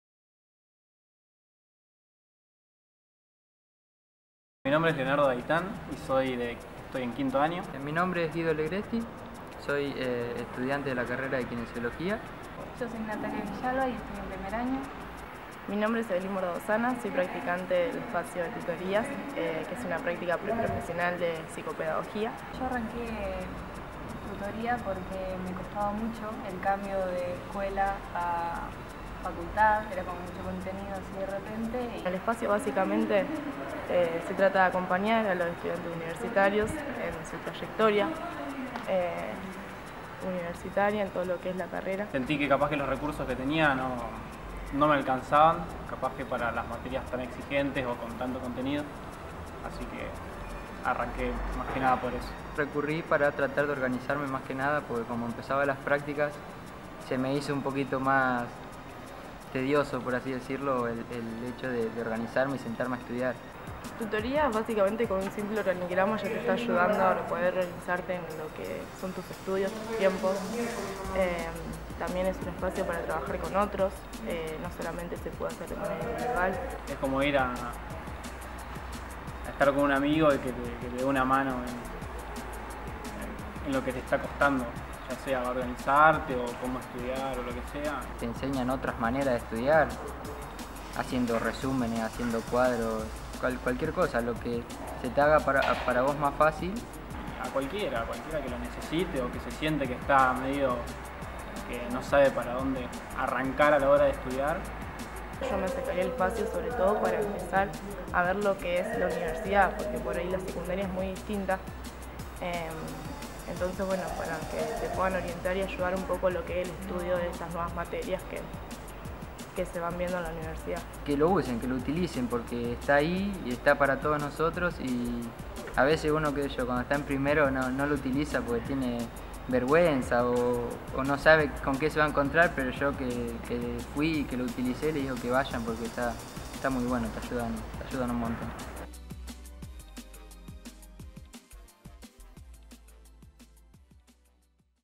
Tutorías y Promoción de la Cultura Universitaria
Desde el área de Tutorías promovemos la hospitalidad institucional de la UGR para contener, alojar y acompañar a los estudiantes en la nueva experiencia universitaria.